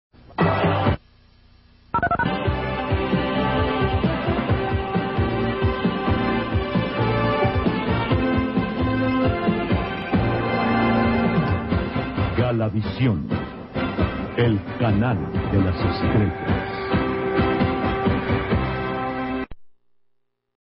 CORTINILLA ID: GALAVISION "EL CANAL DE LAS ESTRELLAS"
Asi lucía la imagen de Galavisión Europa enla década de los noventas.  Al igual que el canal 2 se llamaba "El Canal de las Estrellas".